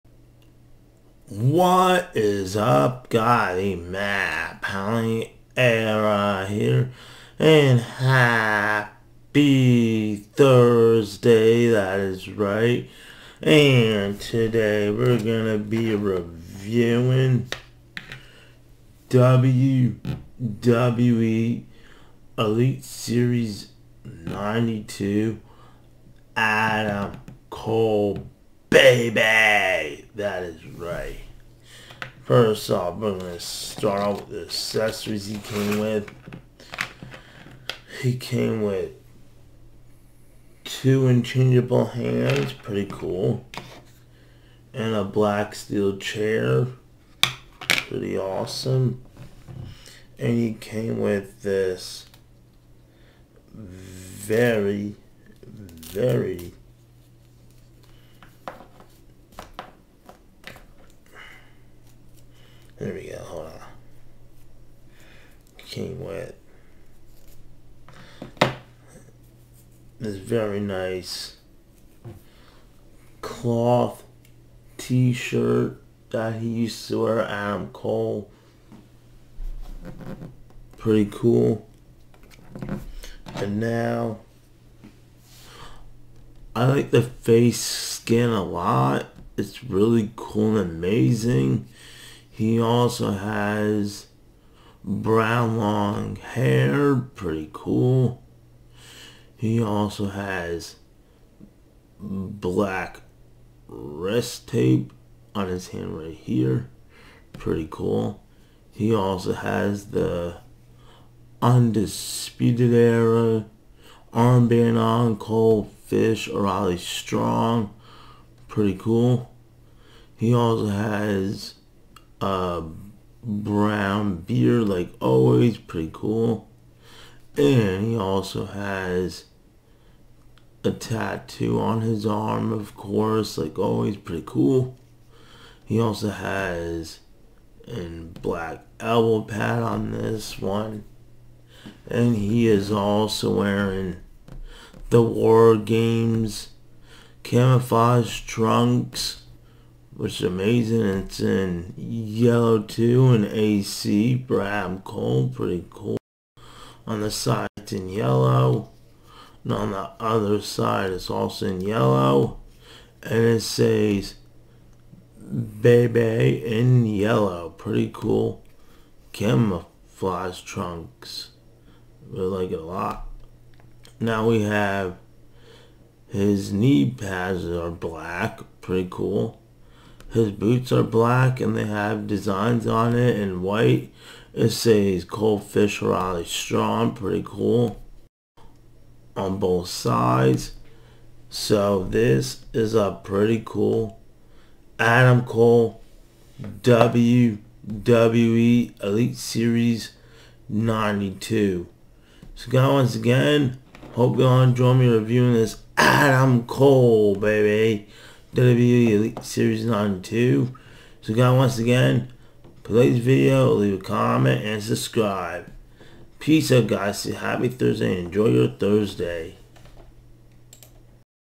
Reviewing WWE Elite Series 92 Adam Cole Figure
So Happy Thursday And Today I Will Be Reviewing My WWE Elite 92 Chase Figure Adam Cole Bay Bay! Enjoy The Review Guys Abd The Video To!